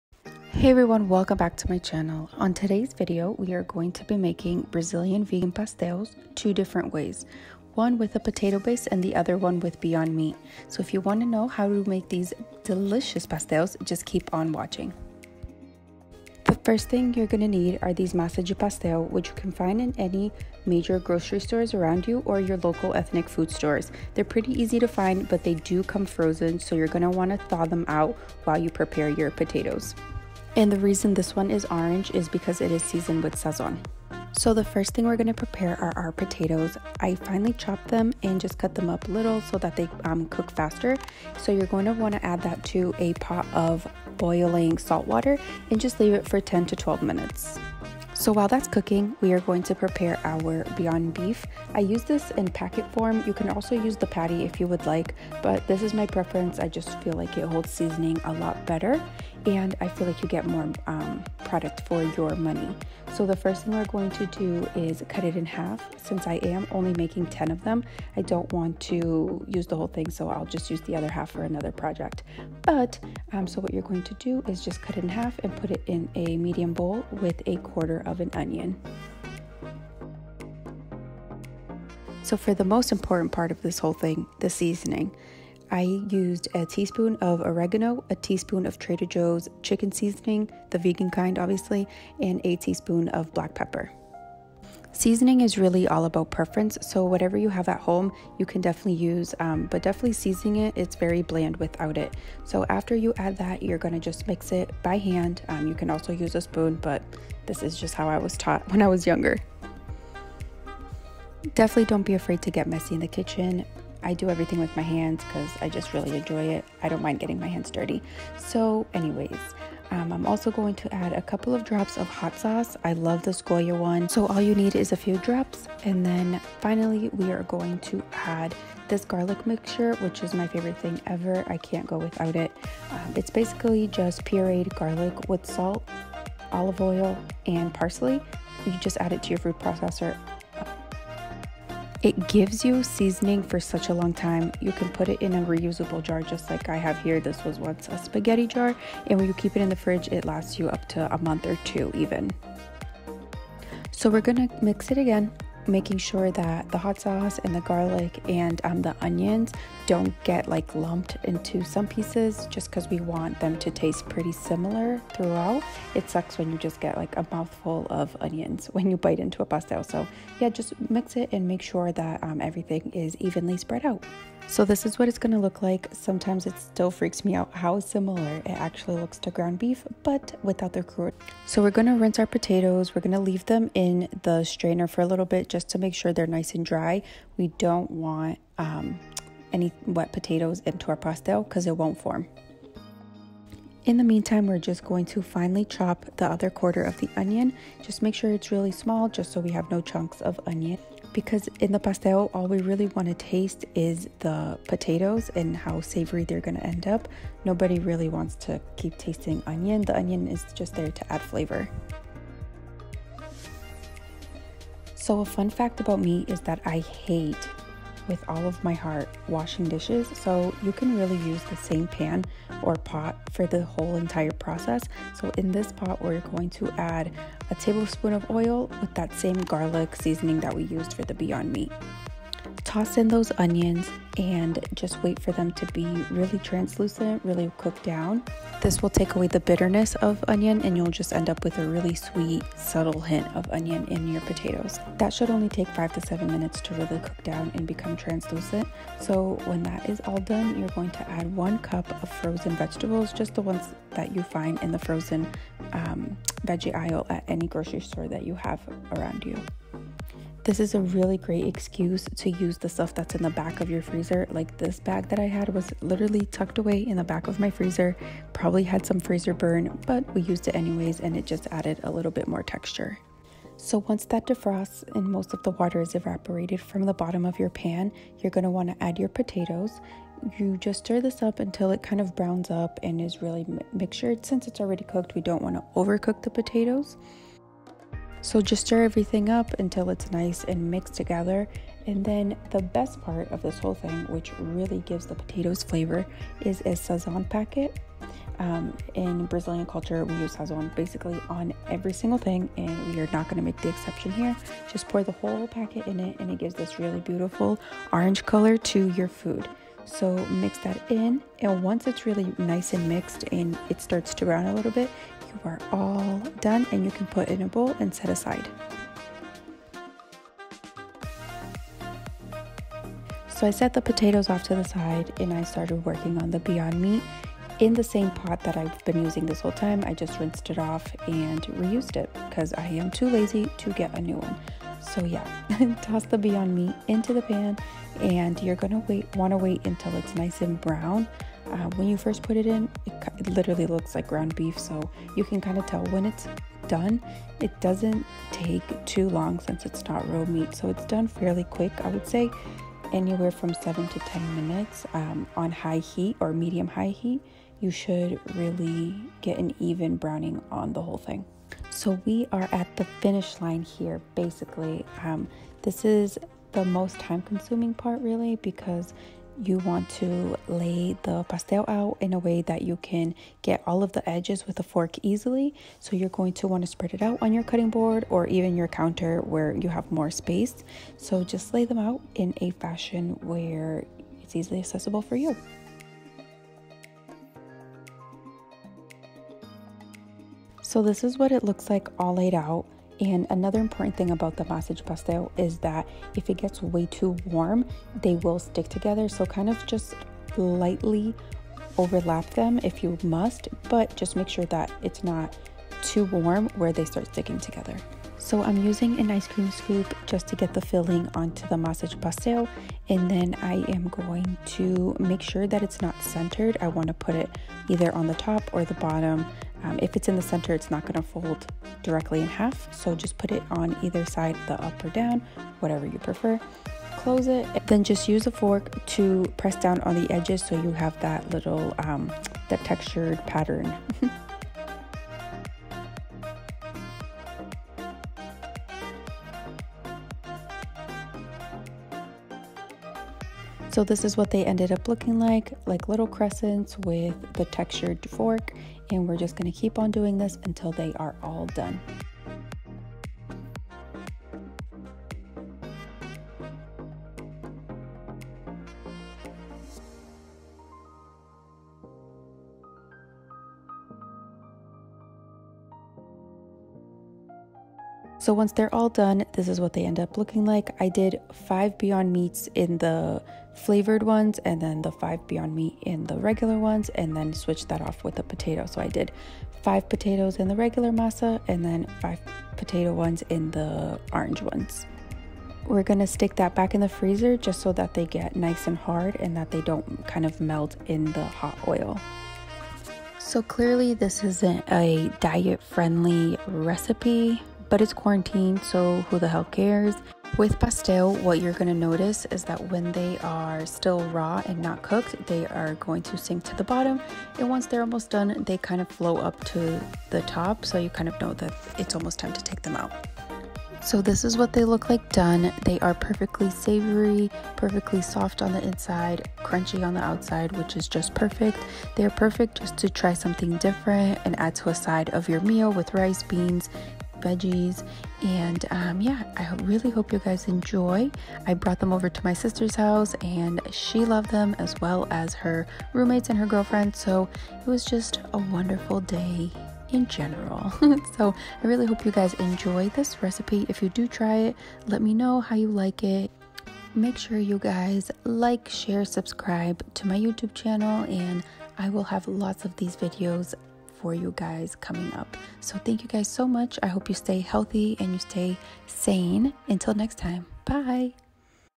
Vegan Brazilian Pastel, Two Ways
Hey everyone welcome to another video! In today's video I made Brazilian pastel two ways, a plant based potato filling and a beyond meat filling. Both came out sooo good! When I went vegan I thought I would have to give up my favorite childhood foods, but I am eating GOODT. I hope you enjoy and make them for yourself!